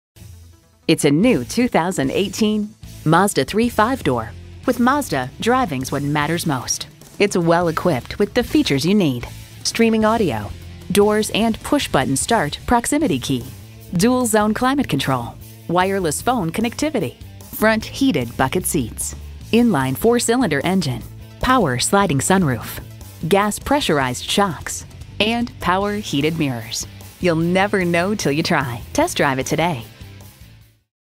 New 2018 Mazda Mazda3 Baltimore, MD 5M851283 - SOLD
Year: 2018
Make: Mazda
Model: Mazda3
Trim: Touring
Engine: 2.5L 4-Cylinder
Transmission: Automatic
Color: Blue
Interior: Black
Stock #: 5M851283
VIN: 3MZBN1L36JM251283

Address:
6616 Baltimore National Pike